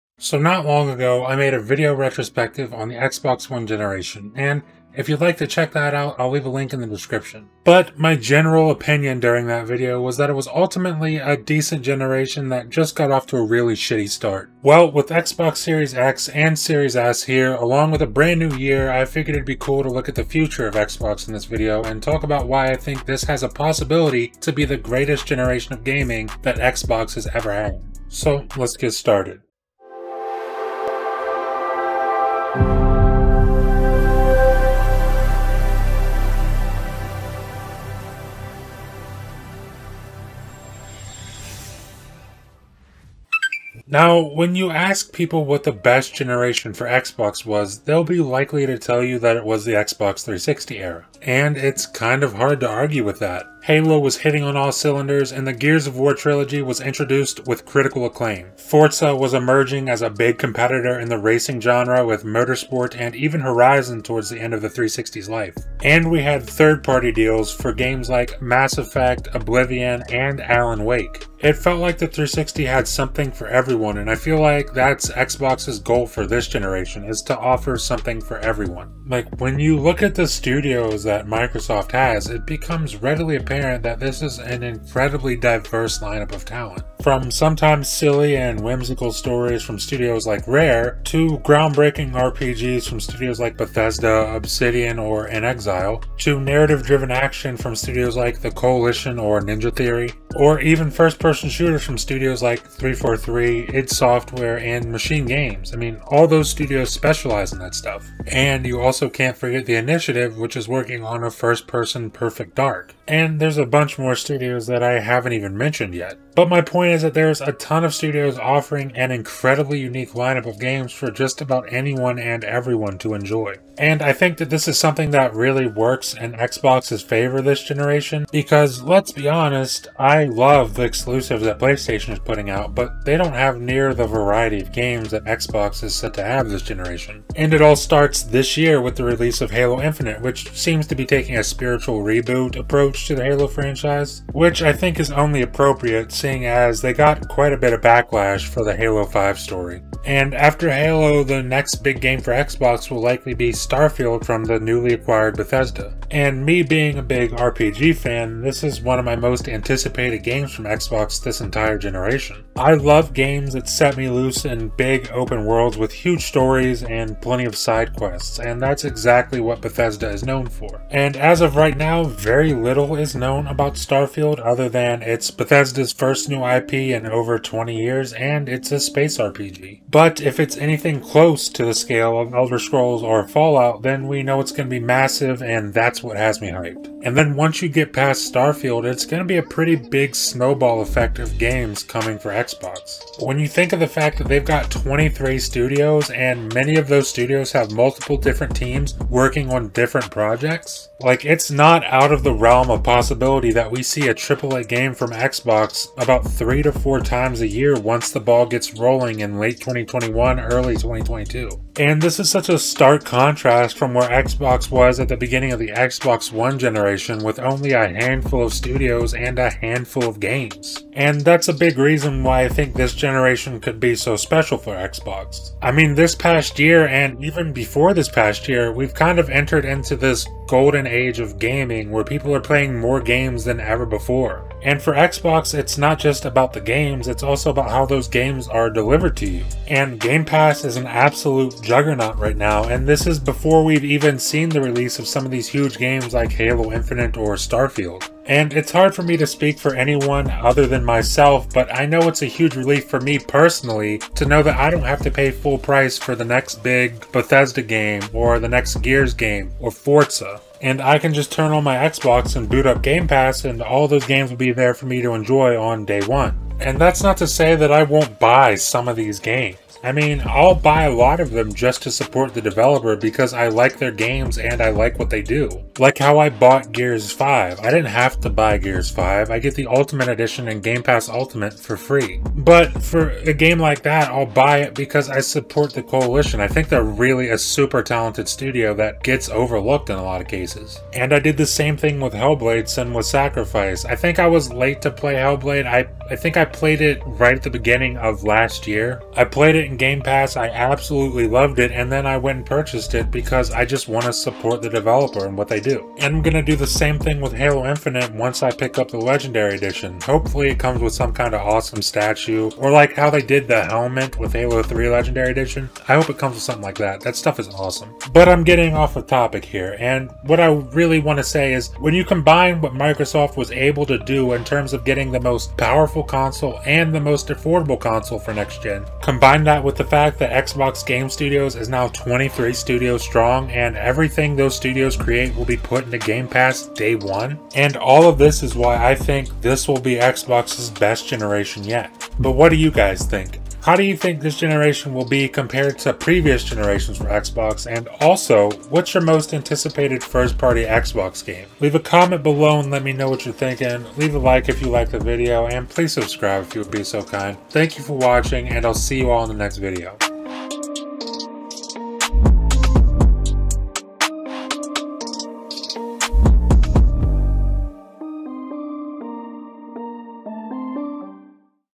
This could be Xbox's Best Generation Yet
In today's video I'm giving my thoughts on why this could be xbox's biggest and best generation yet.